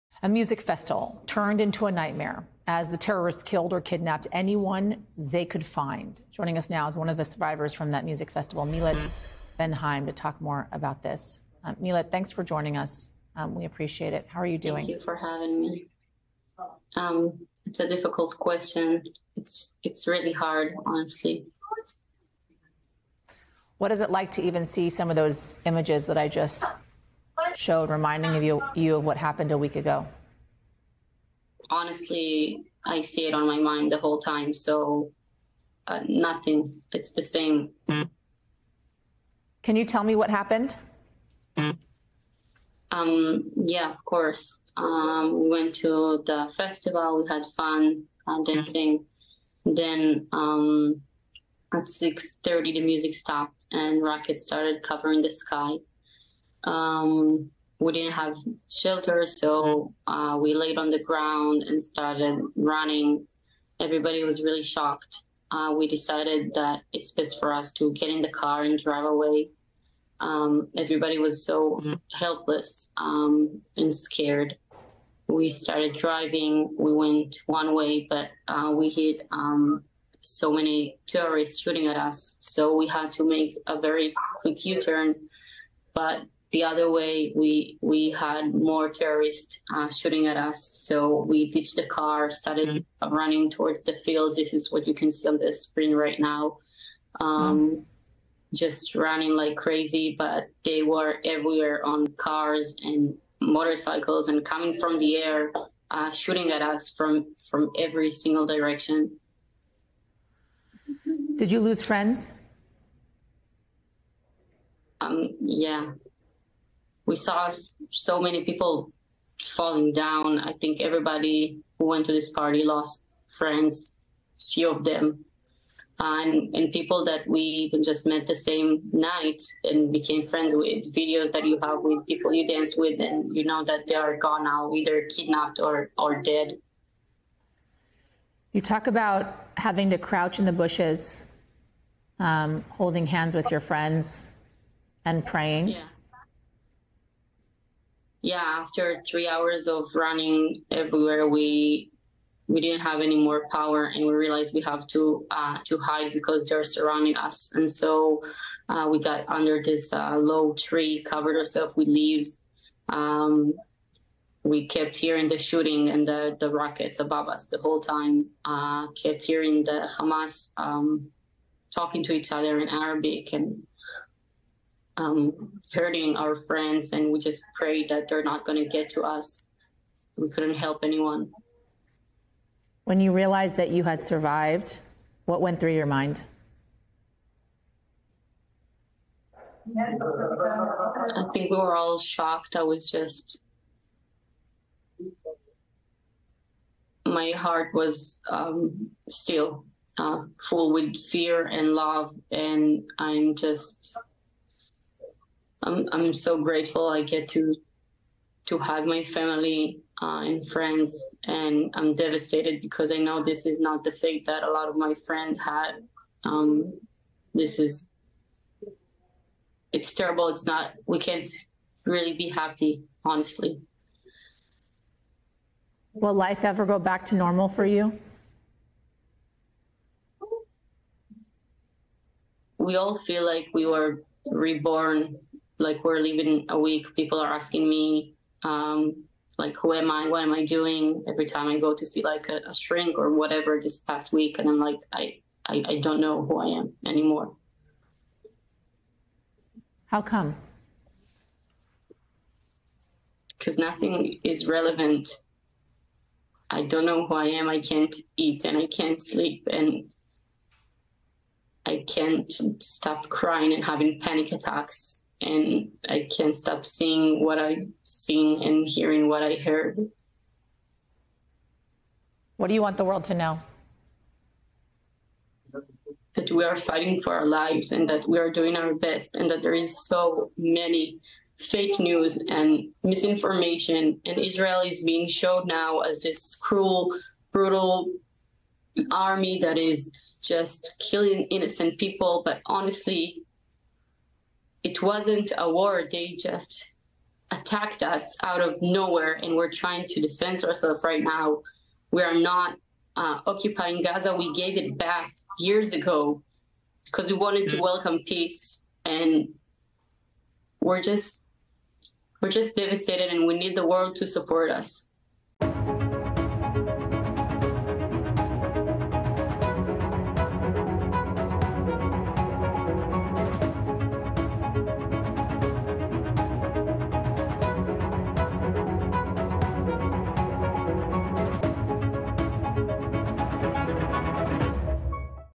Concert survivor on how life has changed since the Hamas attacks
Millet Ben Haim reflects on the past week with MSNBC's Yasmin Vossoughian.